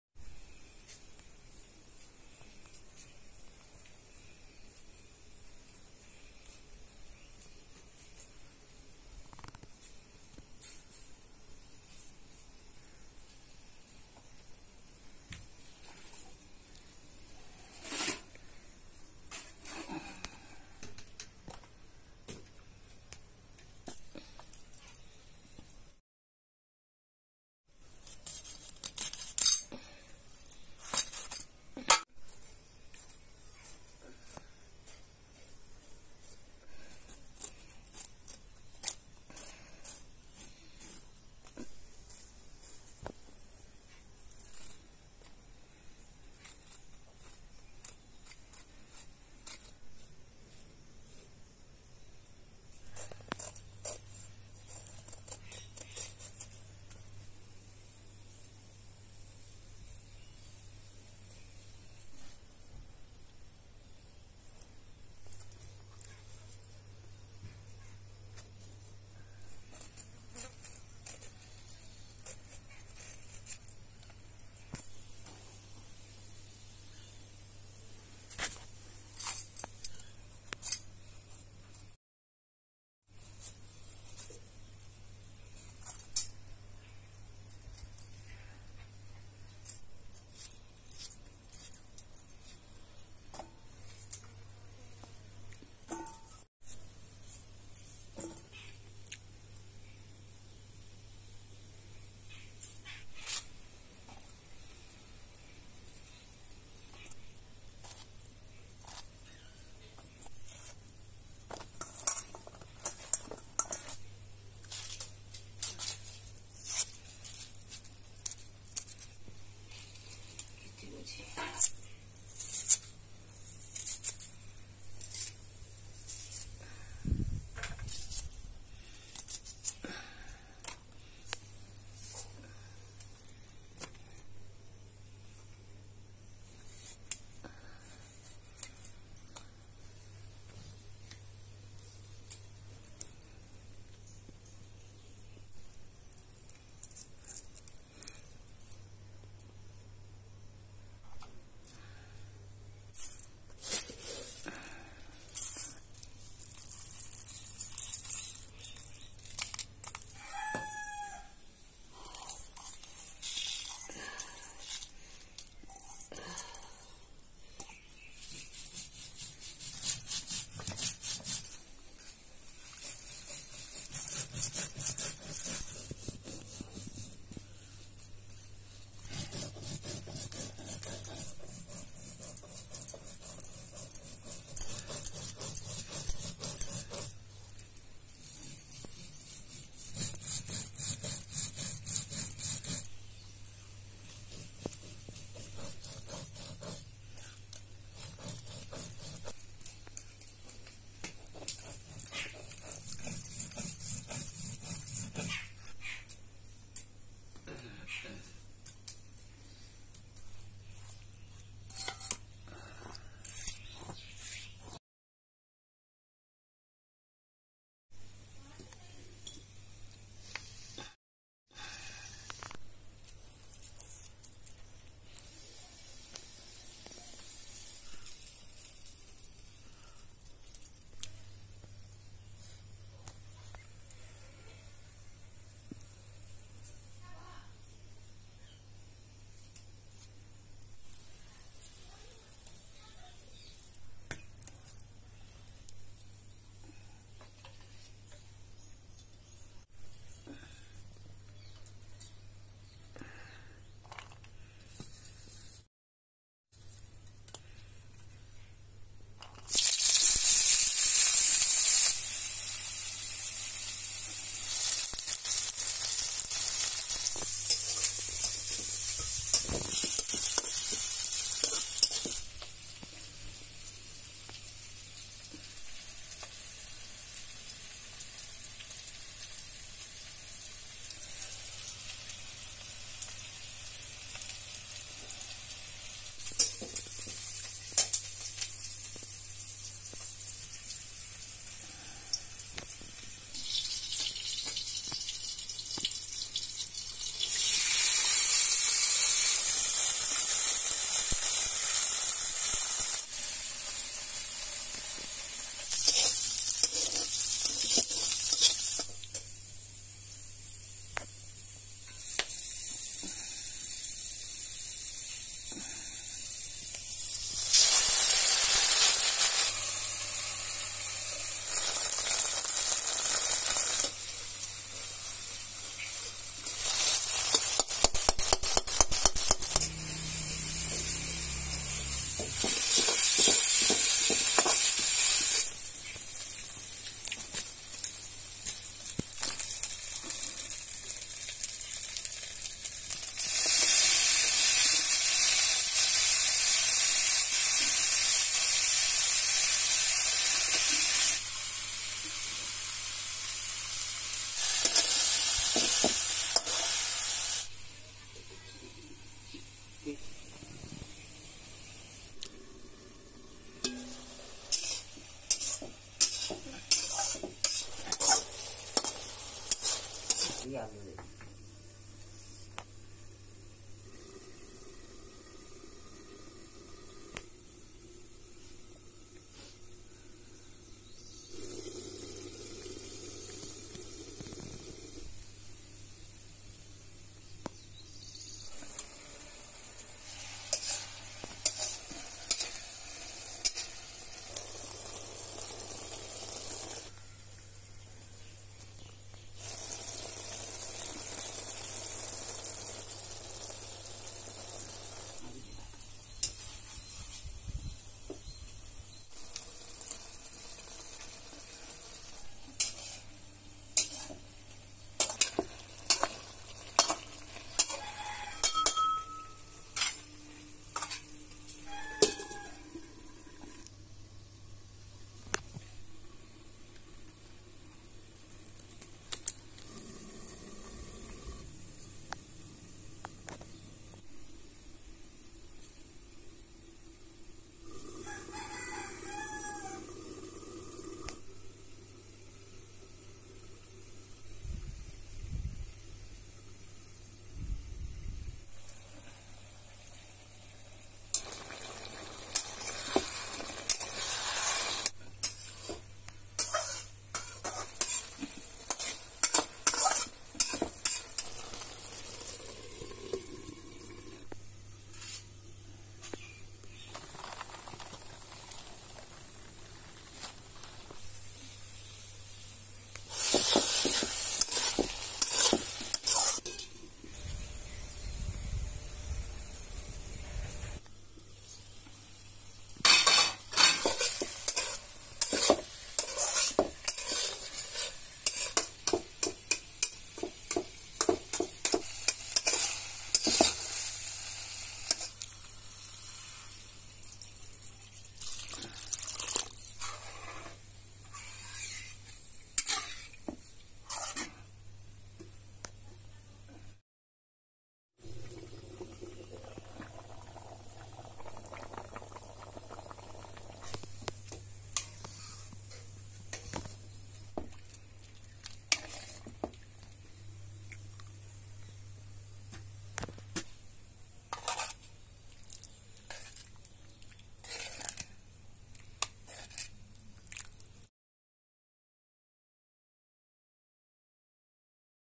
Rural DUNKA Recipe || SPONGE GOURDS Recipe || Deep Rural Cooking
DUNKA ?

Young Sponge gourds may be eaten raw, like cucumbers, or cooked as a vegetable

                                      •(RURAL COOKING)™